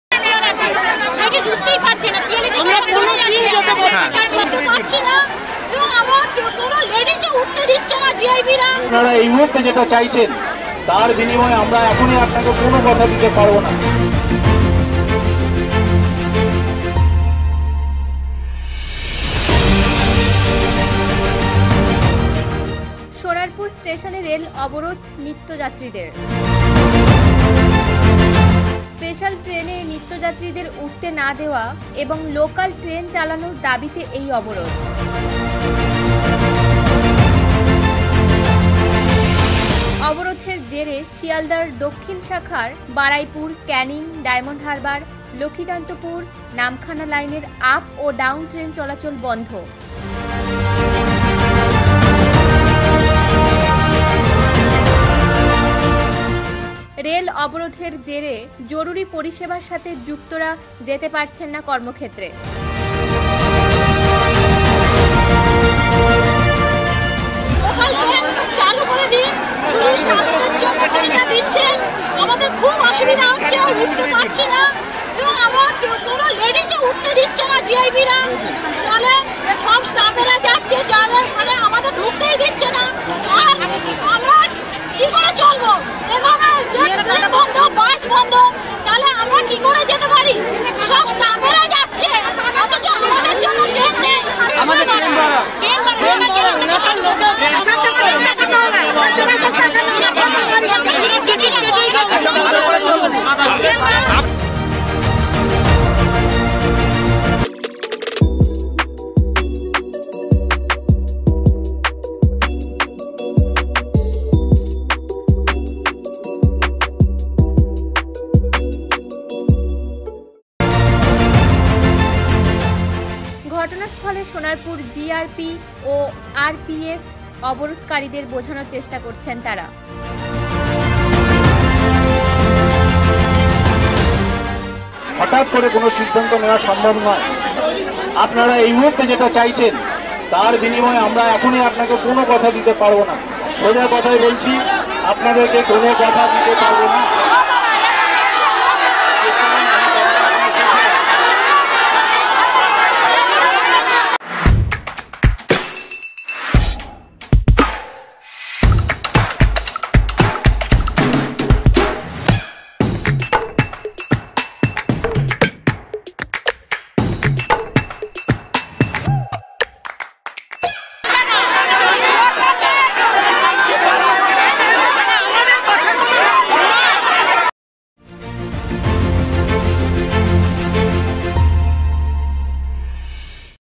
সোনারপুর ষ্টেশনে রেল অবরোধ নিত্যযাত্রীদের.
সোনারপুর ষ্টেশনে রেল অবরোধ নিত্যযাত্রীদের। স্পেশাল ট্রেনে নিত্য যাত্রীদের উঠতে না দেওয়া এবং লোকাল ট্রেন চালানোর দাবিতে এই অবরোধ। অবরোধের জেরে শিয়ালদহ দক্ষিন শাখার বারুইপুর, ক্যানিং, ডায়মন্ডহারবার, লক্ষ্মীকান্তপুর, নামখানা লাইনে আপ ও ডাউন ট্রেন চলাচল বন্ধ। রেল অবরোধের জেরে জরুরি পরিষেবার সাথে যুক্তরা যেতে পারছেন না কর্মক্ষেত্রে। ঘটনাস্থলে সোনারপুর GRP ও RPF। অবরোধকারীদের বোঝানোর চেষ্টা করছেন তারা।